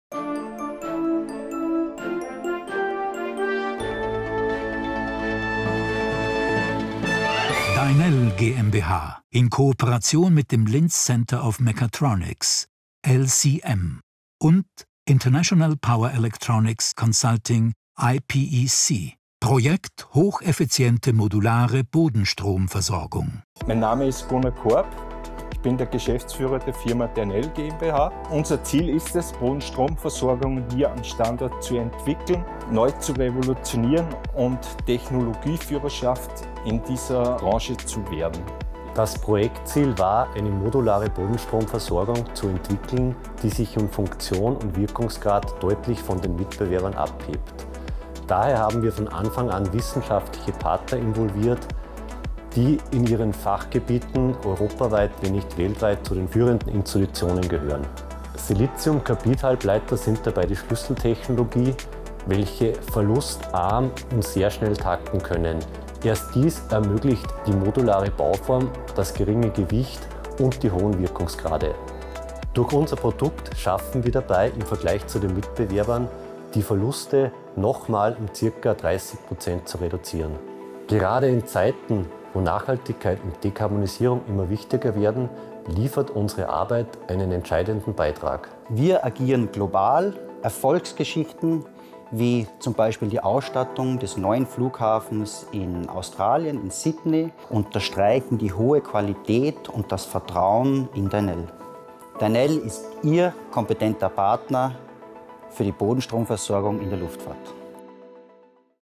Hocheffiziente modulare Bodenstromversorgung | VERENA Award
Wenn ein Flugzeug nicht fliegt, kann es genauso gut Energie sparen: Eine oberösterreichische Firma konkurriert auf dem Weltmarkt mit ihrem praktischen Bodenstromaggregat. Dynell GmbH in Kooperation mit dem Linz Center of Mechatronics (LCM) und International Power Electronic Consulting (IPEC) haben dieses Projekt gemeinsam entwickelt.